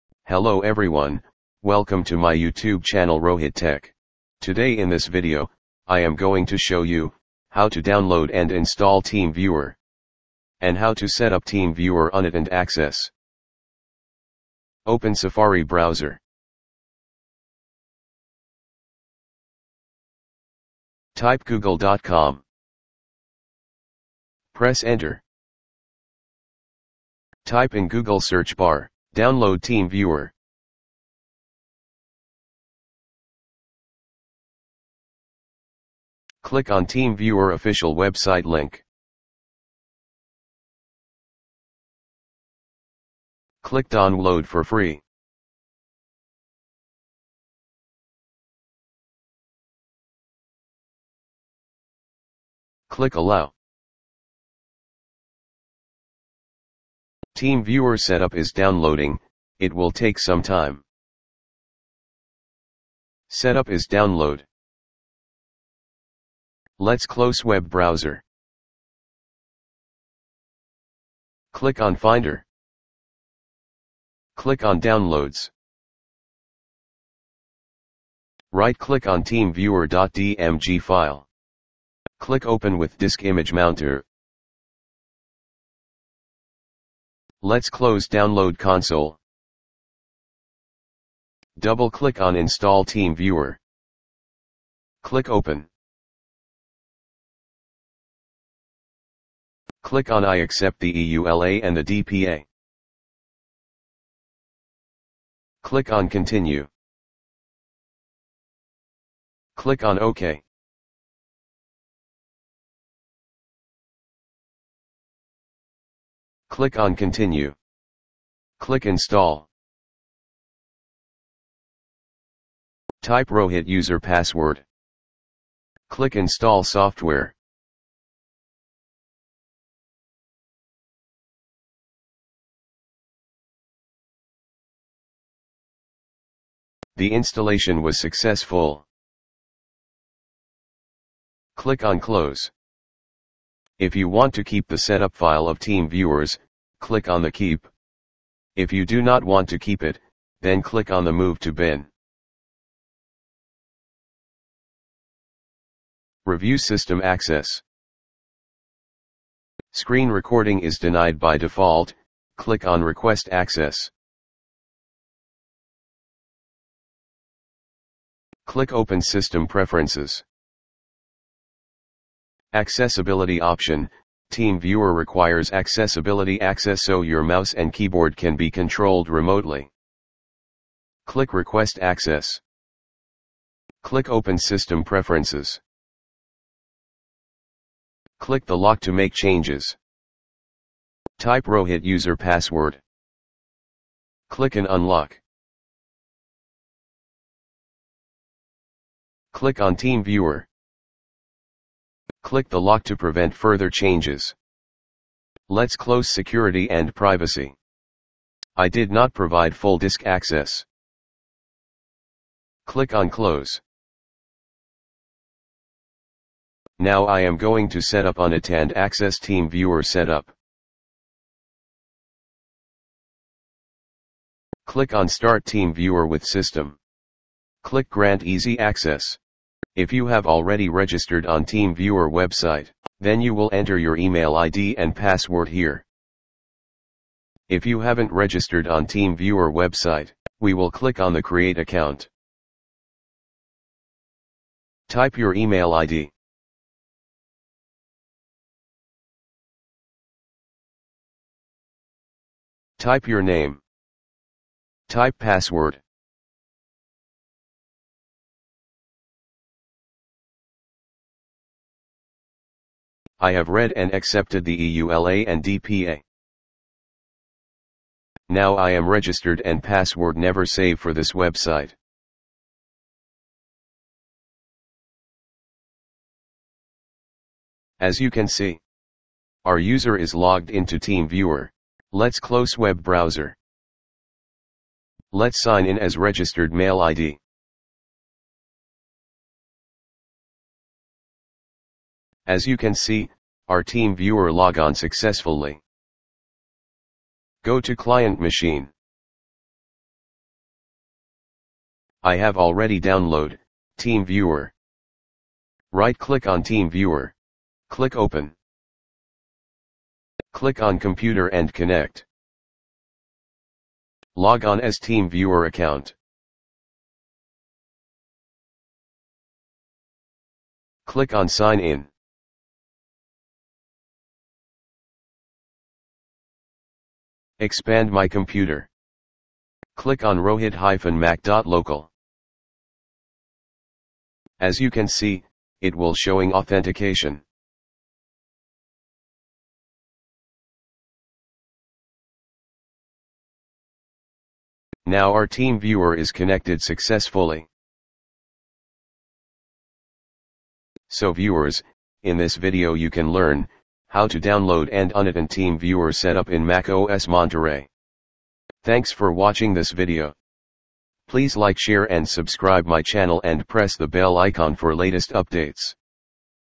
How to Install TeamViewer (2022) on macOS Monterey !! How to Setup Unattended Access on TeamViewer
Hello Everyone
!! How to Download & Install TeamViewer (2022) on macOS Monterey !!
!! How to Setup Unattended Access on TeamViewer !!